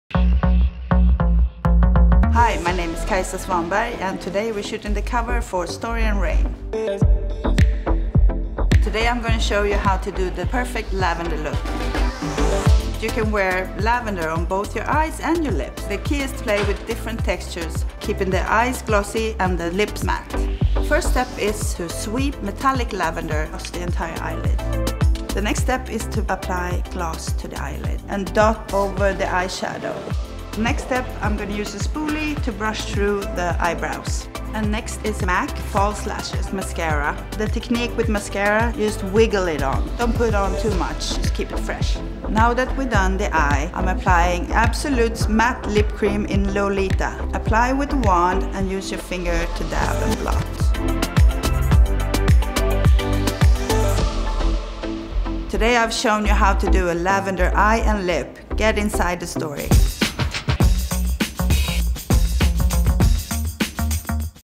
The Lavender Look by Kajsa Svanberg ft. Lauren Layne for Story + Rain
Master a lavender look on your lids and lips in this exclusive hair tutorial featuring model Lauren Layne for StoryandRain.com.